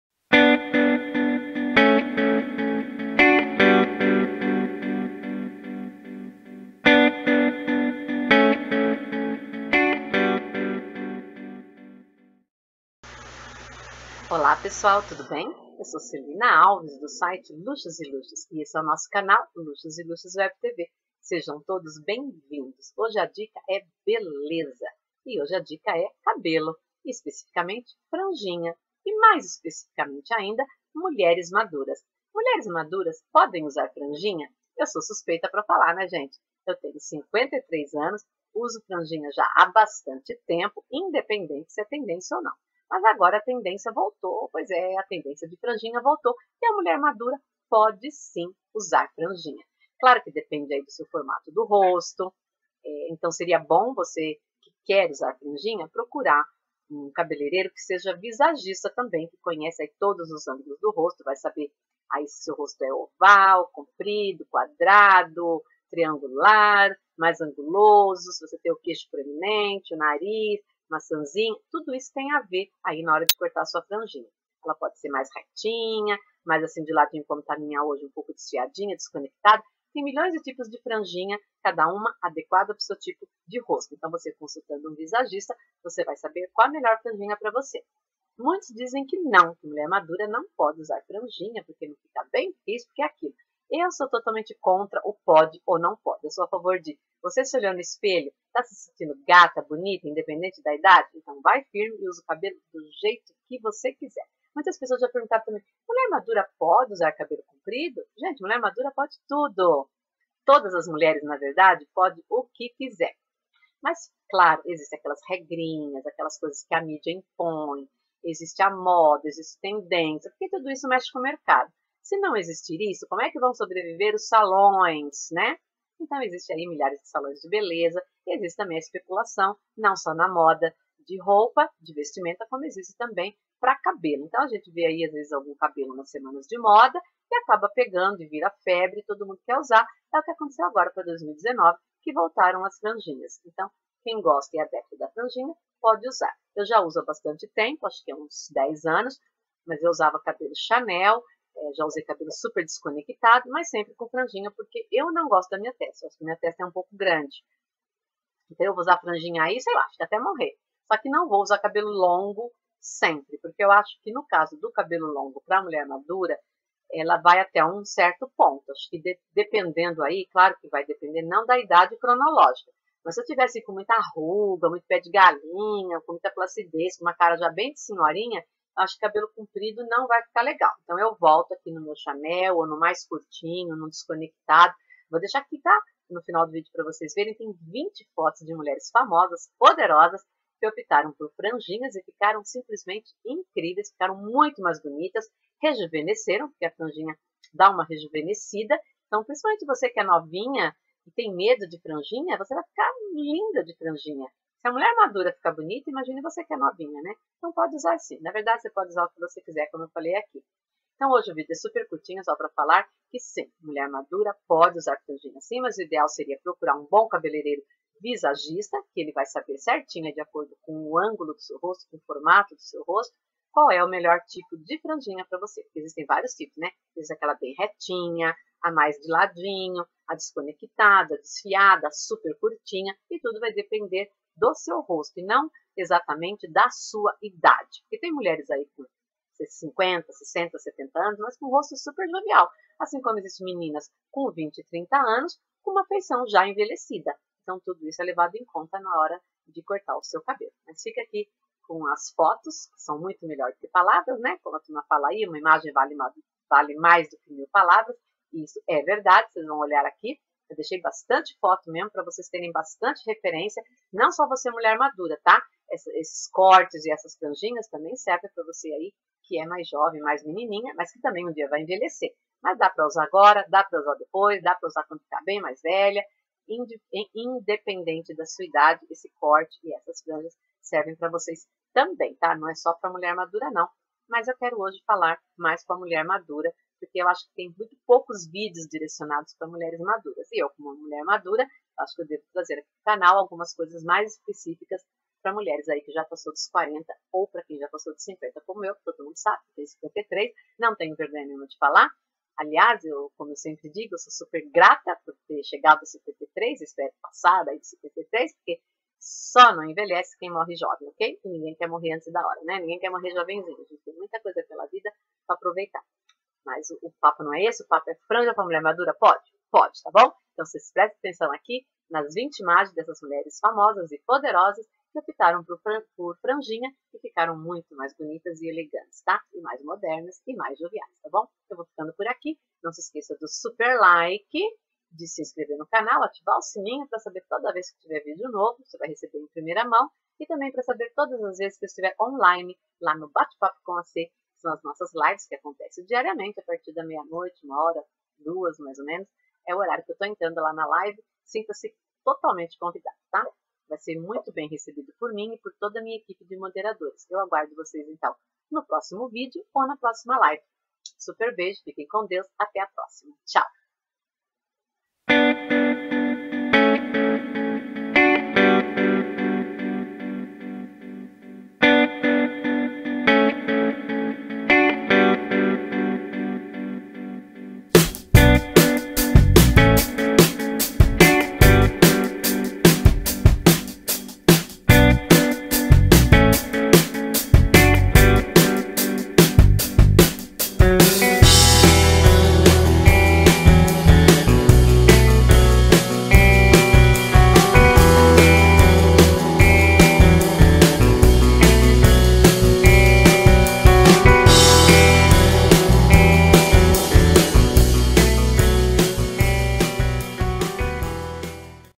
Mulheres Maduras Podem Usar Franjinha?
Tendência de cabelos para 2019 são as franjinhas, mas será que mulheres maduras podem usar? É aí que entra o trabalho do hair stylist visagista.